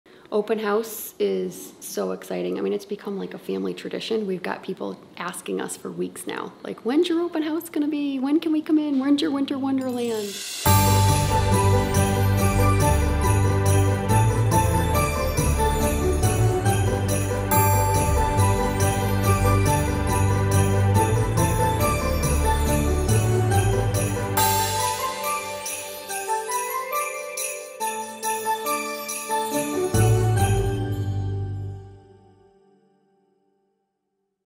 Our Winter Wonderland is Here
It’s the moment you’ve been waiting for – our Open House Event is all week long! Our store is completely decorated. Every nook and cranny with a new holiday décor item. Come explore our high quality furniture items and get swept away by the magic of our holiday flare. See you soon at Kloter Farms!